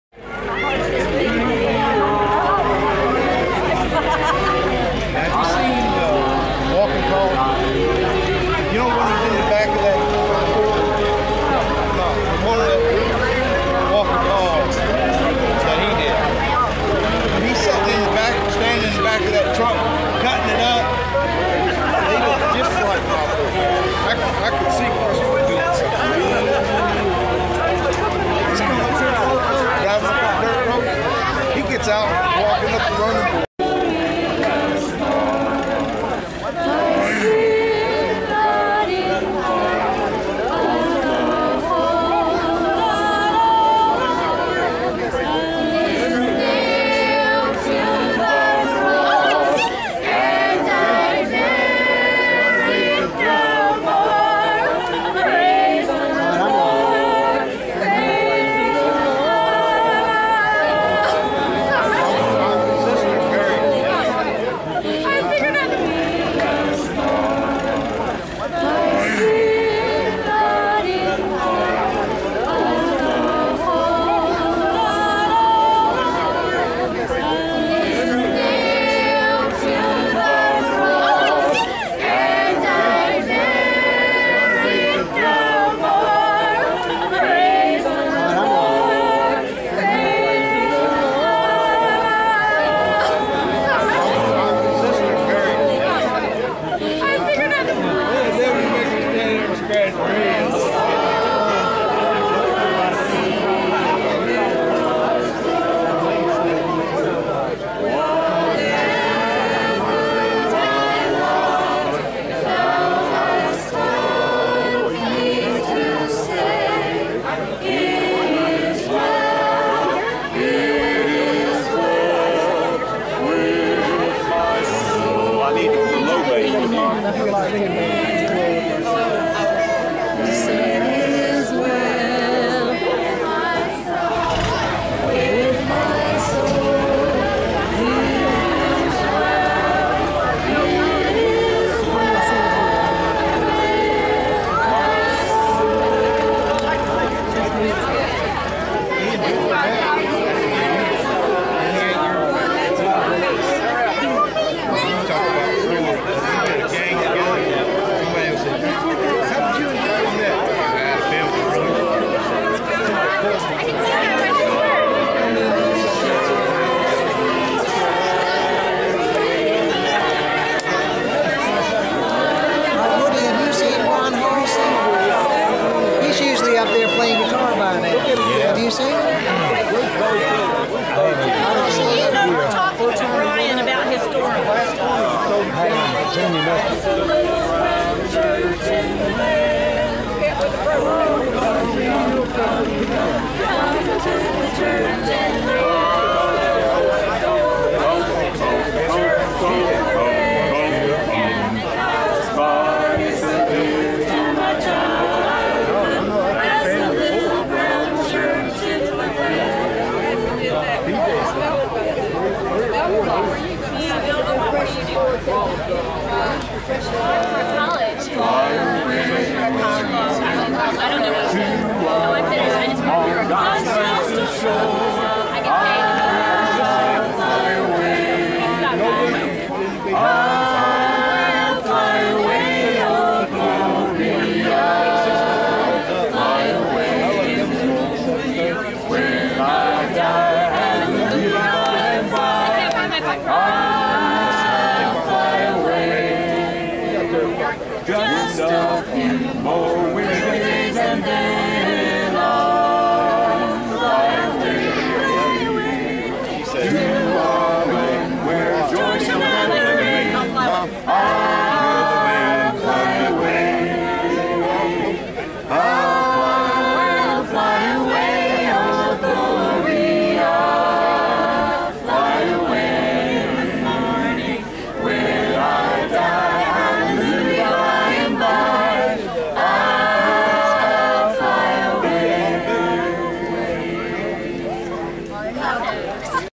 Houck Reunion Halsey singers
Houck Reunion, Mary houck halsey family singing hymns. at twin falls state park, W.VA